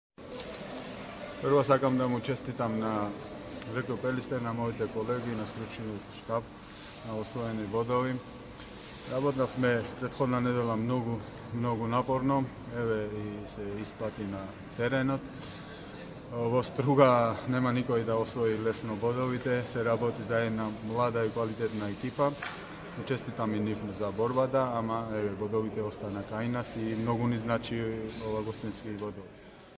Pelister HC - Revaz Chanturija best goal scorer vs Struga
Player of the Match against Struga , Revaz Chanturija 8 goals , Pelister HC - RK Struga .